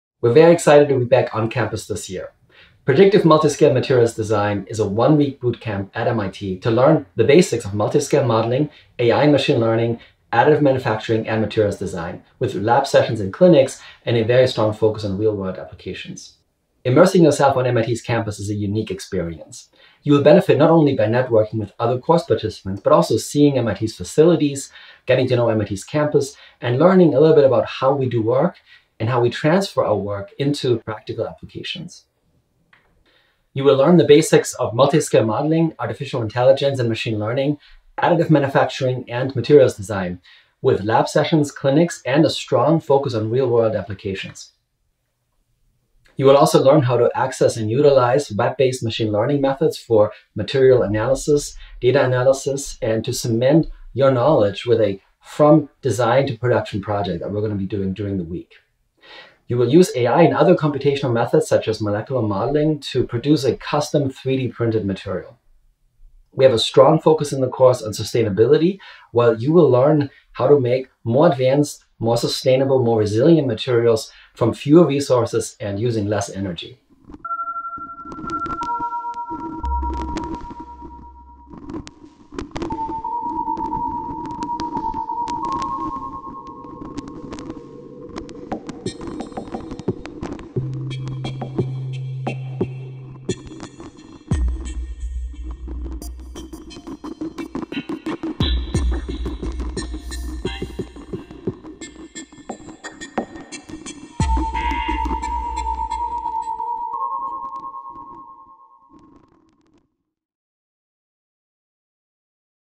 MIT Short Course: Predictive Multiscale Materials Design
In a time of constant change, build security into your future by learning intelligent material design skills necessary for innovation.  We at MIT, welcome you to join us this summer, in person, for a hands-on design-to-product Predictive Multiscale Materials Design course from June 13-17, 2022.  All course registrants will receive an MIT certificate upon completion.